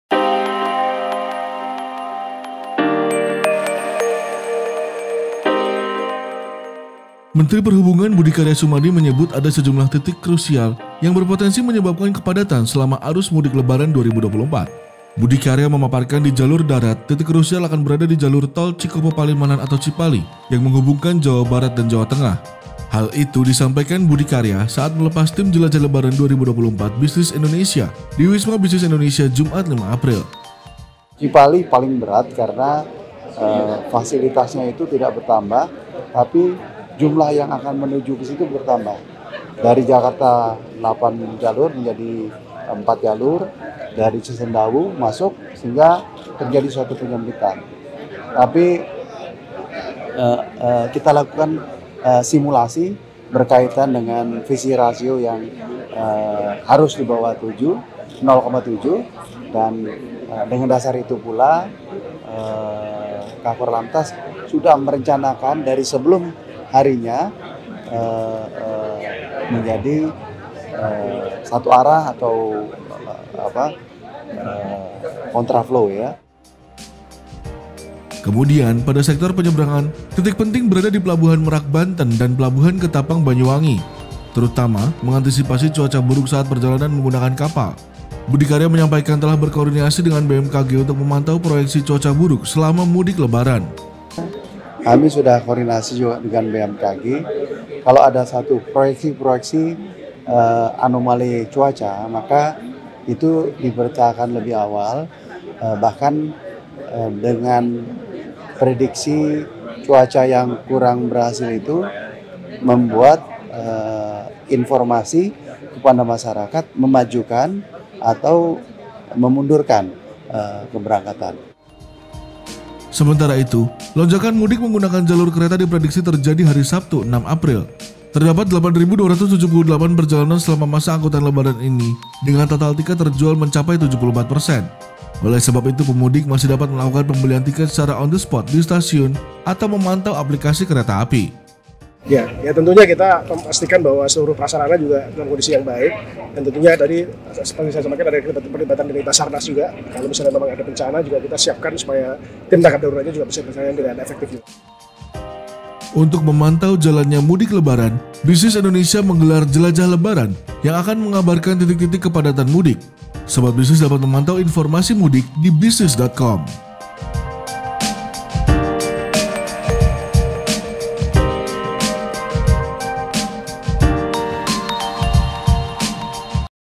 Tiga Titik Krusial Mudik Cipali, Merak, dan Ketapang, Mengapa?
Menteri Perhubungan Budi Karya Sumadi menyebut ada sejumlah titik krusial yang berpotensi menyebabkan kepadatan selama arus mudik Lebaran 2024. Budi Karya memaparkan, di jalur darat, titik krusial akan berada di jalur Tol Cikopo-Palimanan (Cipali), Pelabuhan Merak Banten, dan Pelabuhan Ketapang Banyuwangi.